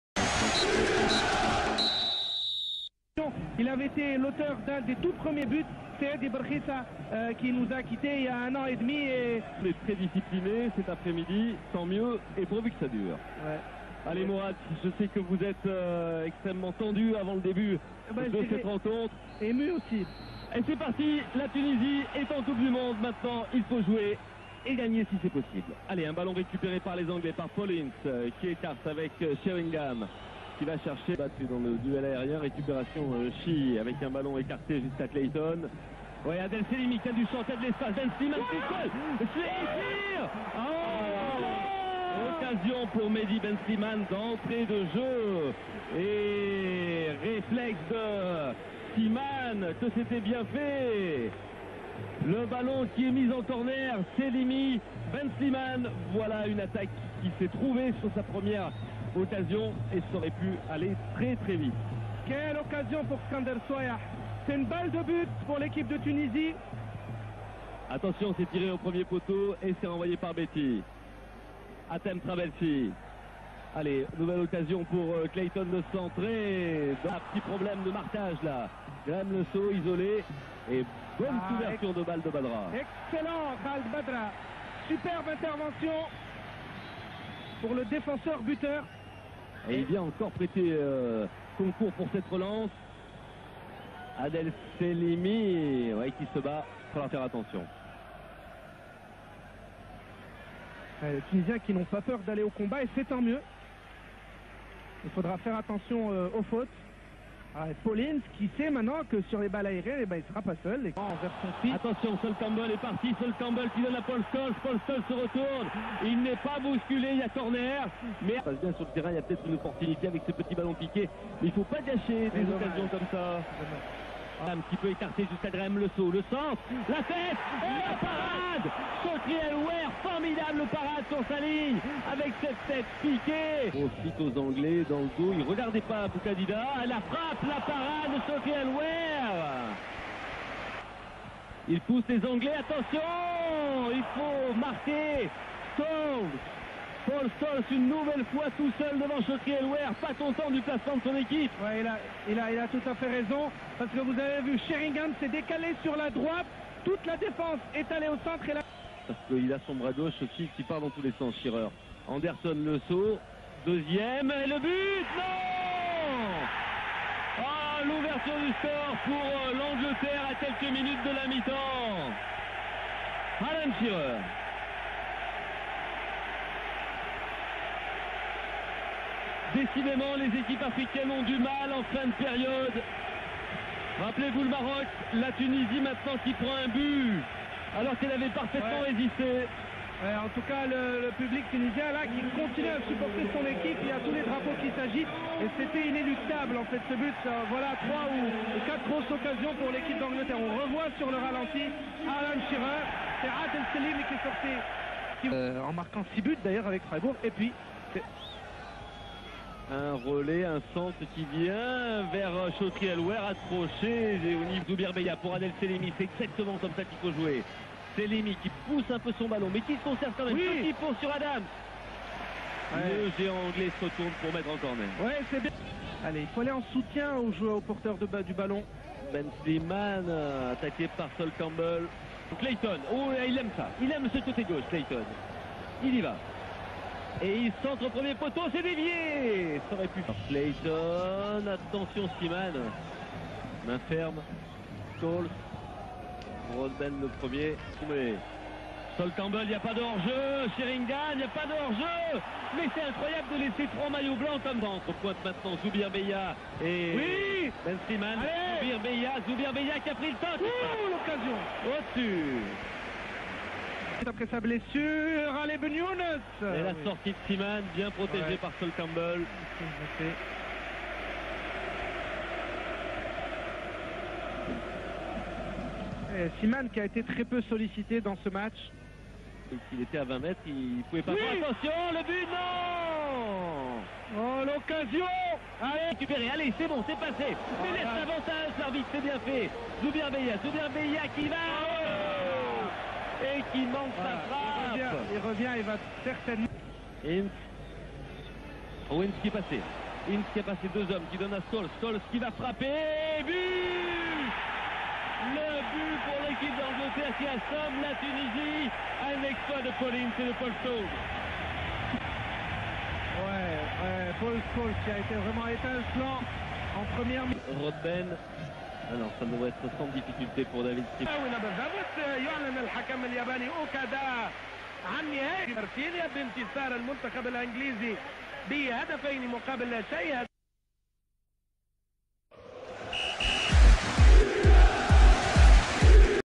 Tunisia 0-2 England 1998 [Canal+ Horizons]
Lundi 15 juin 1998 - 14h30
Coupe du Monde France 1998 | Poules | Groupe G
Stade Vélodrome (Marseille / France)
Arbitre : Masayoshi Okada (Japon)
Angleterre vs Tunisie (2-0) (انجلترا تونس 0-2)

Les buts marqués :
Alan Shearer (43')
Paul Scholes (89')

Les Formations :

Les Aigles de Carthage :
Chokri El Ouaer , Hatem Trabelsi (79') , Sami Trabelsi , Mounir Boukadida , Khaled Badra , José Clayton , Kaïs Ghodhbane ,  Sirajeddine Chihi , Skandar Souayah (46') , Adel Sellimi , Mehdi Ben Slimane (65')

Remplaçants :
Tarek Thabet (79') , Zoubaïer Baya (46'), Imed Ben Younes (65'), Radhouane Salhi , Ali Boumnijel , Ferid Chouchane , Sabri Jaballah , Mourad Melki ,  Riadh Bouazizi , Fayçal Ben Ahmed , Riadh Jelassi

Entraîneur: Henryk Kasperczak (Pologne / France)

The Three Lions :
David Seaman , Sol Campbell , Graeme Le Saux , Tony Adams ,  Gareth Southgate , Paul Ince , David Batty , Paul Scholes , Alan Shearer , Teddy Sheringham (85'), Darren Anderton

Remplaçants :
Michael Owen (85') , Nigel Martyn , Tim Flowers , Gary Neville , Martin Keown , Rio Ferdinand , David Beckham , Steve McManaman , Rob Lee , Paul Merson

Entraîneur: Glenn Hoddle (United Kingdom)